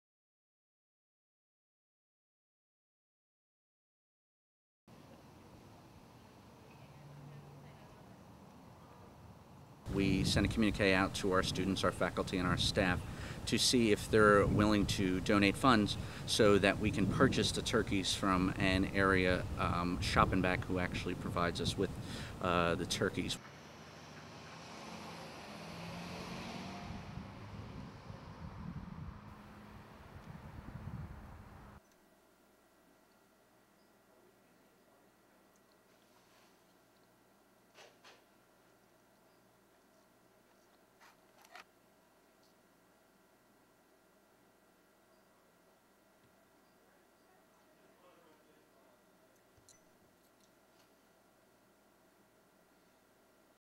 Ambler Turkey Drive VOSOT
There are many families in need of a good meal this Thanksgiving. Ambler campus students, faculty and staff are doing their part to help.